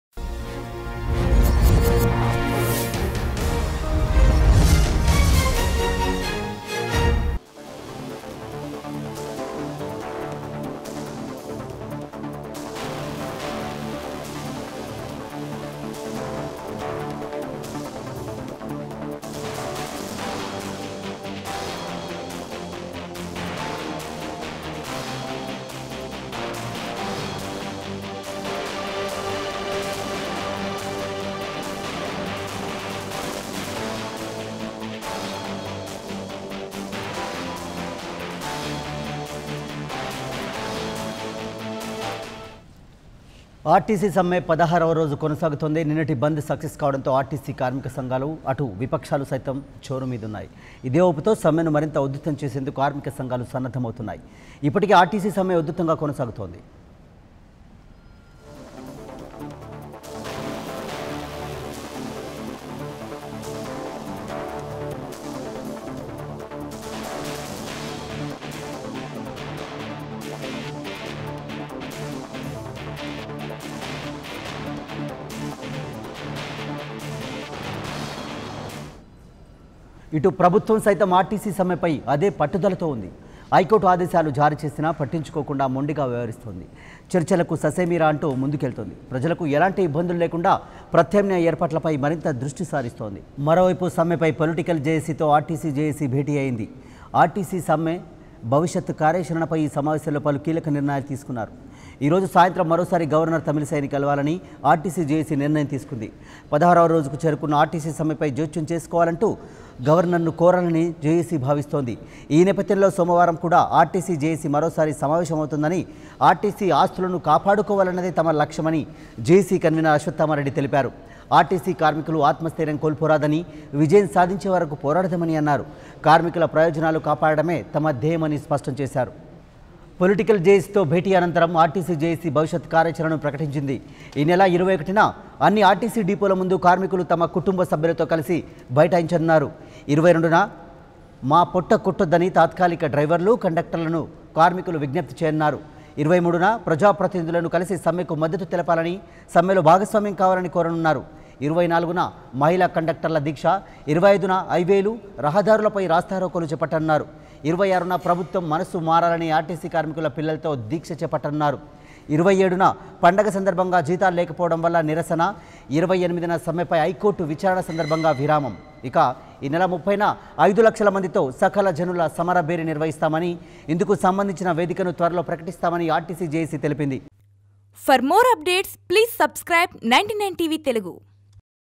16వ రోజుకి చేరిన ఆర్టీసీ సమ్మె | Telangana RTC Latest Updates | 99 TV Telugu
Telangana RTC Latest Updates. TSRTC leaders latest news.